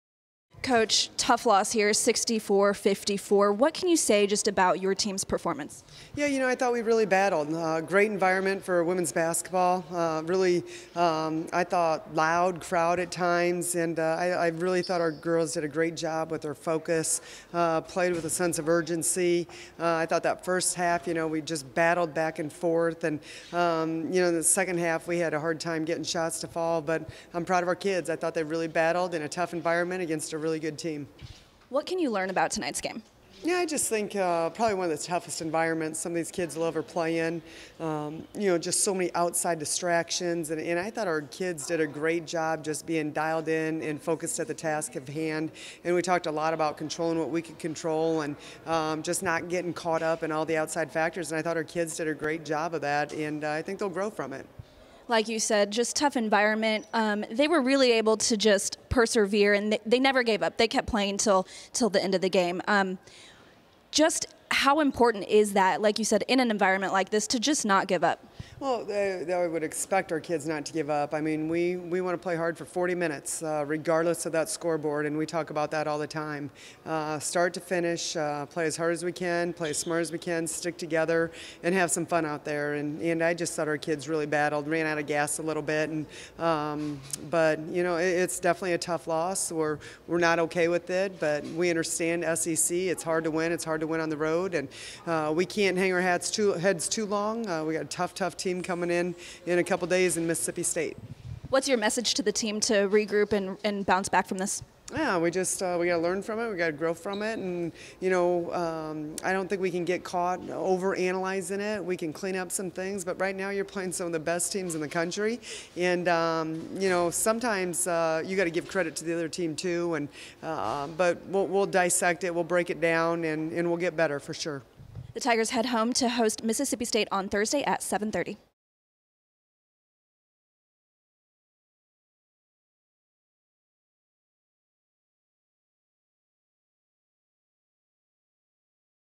POSTGAME INTERVIEW: Pingeton on Loss at South Carolina
Hear from MizzouWBB head coach Robin PIngeton after the Tigers' fall to South Carolina on Sunday. This is a production of the Mizzou Network.